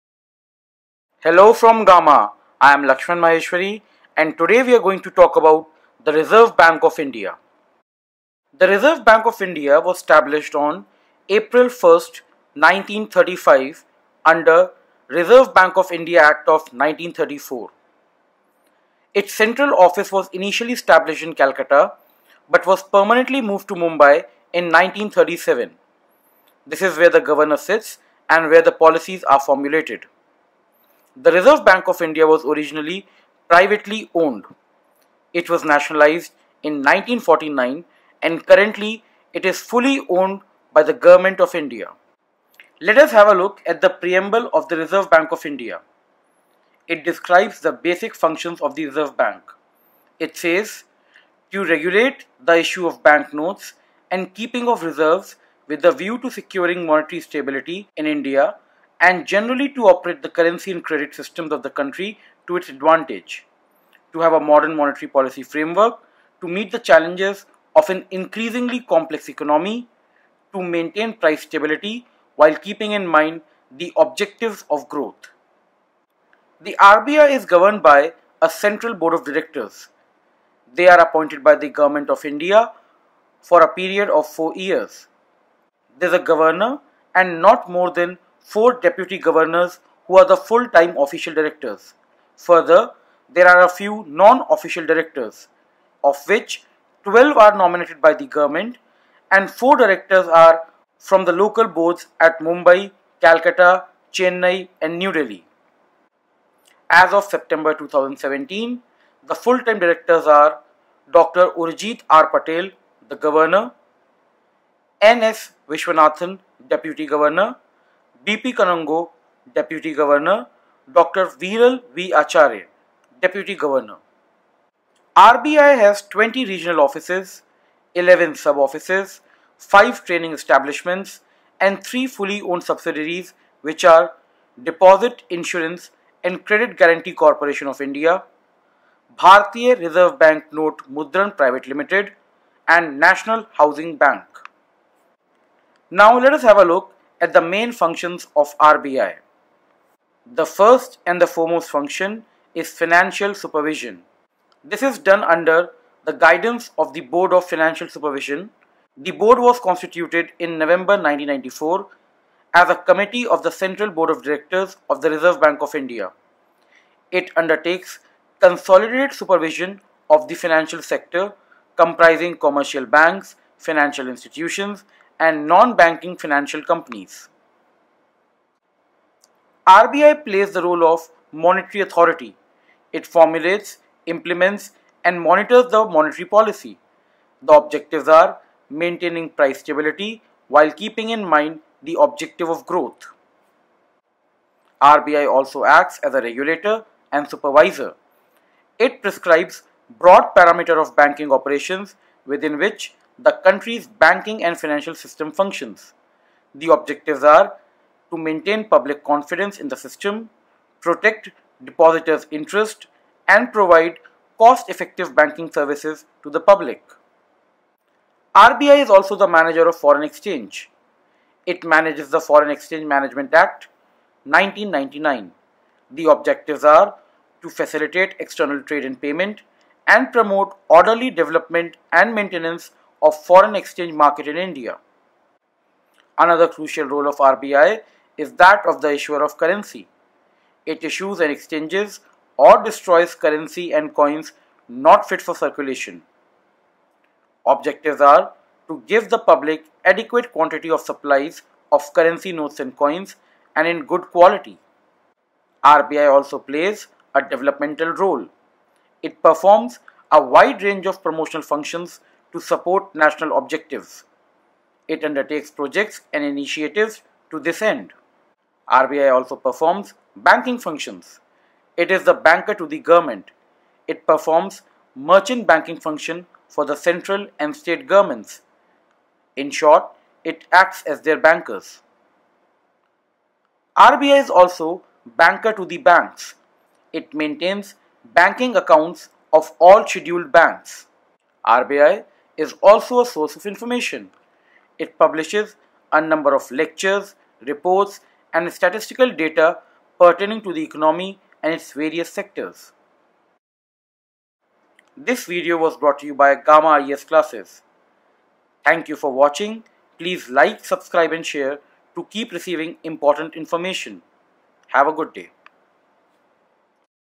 The Reserve Bank of India | India's Central Bank | Complete coverage - By Lakshman Maaheshwary
Know about the Central Bank of India: RBI. Know its functions and responsibilities.